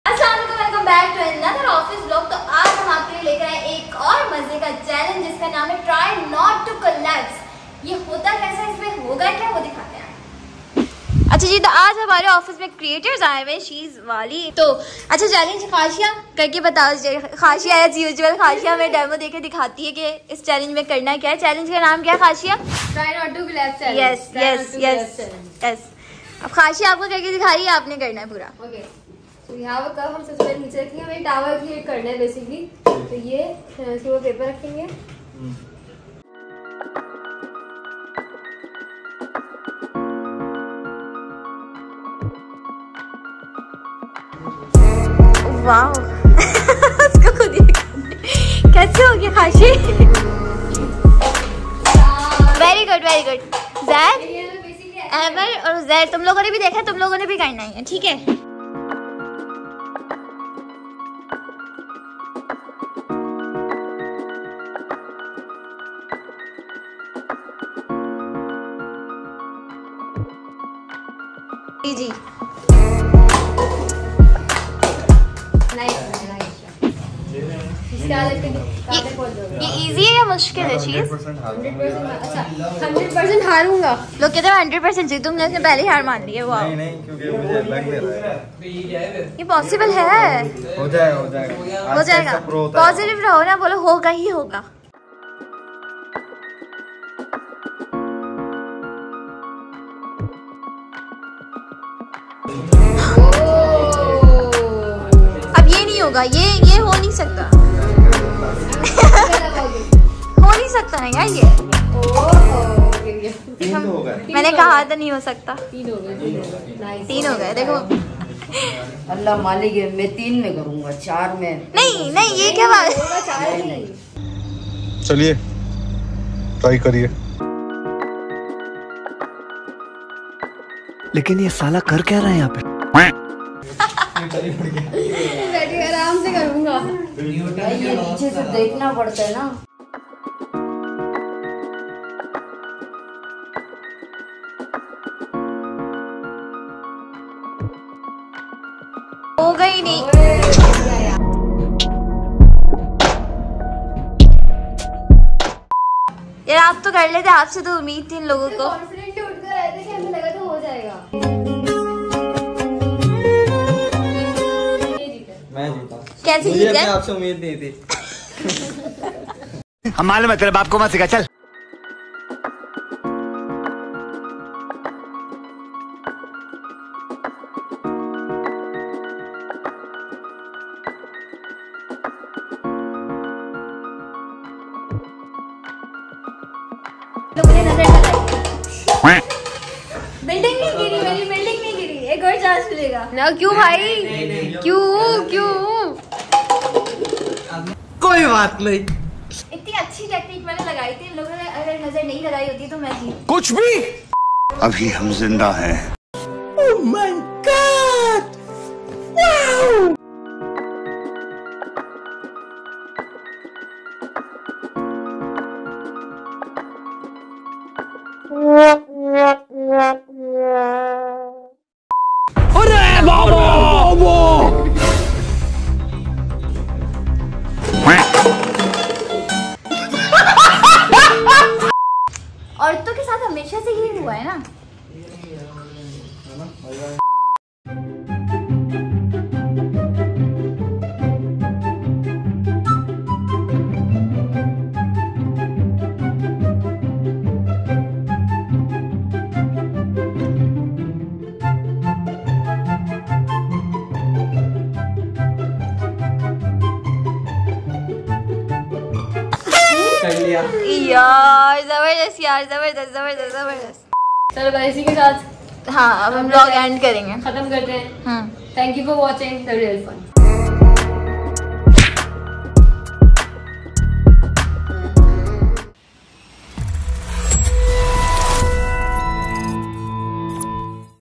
Try Not To Collapse Challenge with @SheesWali | Office Vlog | The Real Fun
In this vlog, we take on an exciting and hilarious game with our fellow creator friends called the Try Not To Collapse the plastic cups challenge! Tune in to find out who comes out on top in this fun-filled challenge!